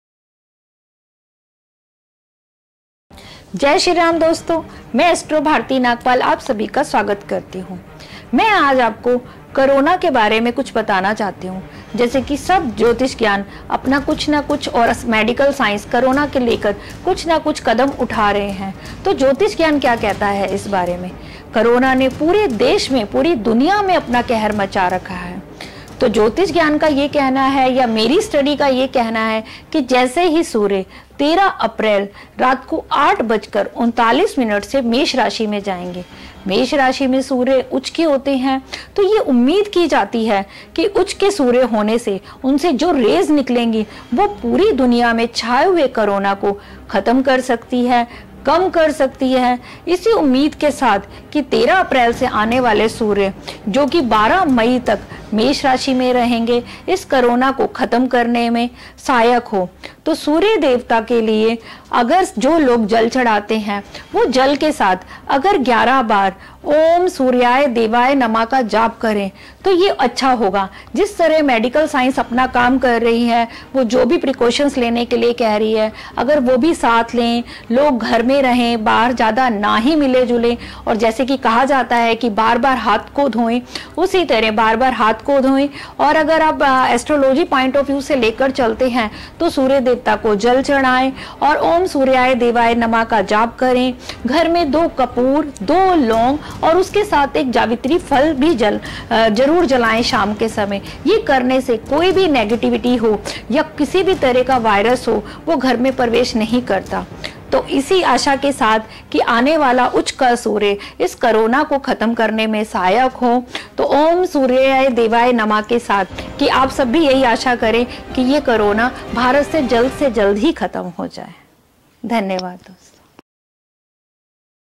क्या सूर्य का मेष राशि में प्रवेश कोरोना वायरस को खत्म करेगा ?
Talk to the Astrologer Bharti Nagpal and ask for personalized solution.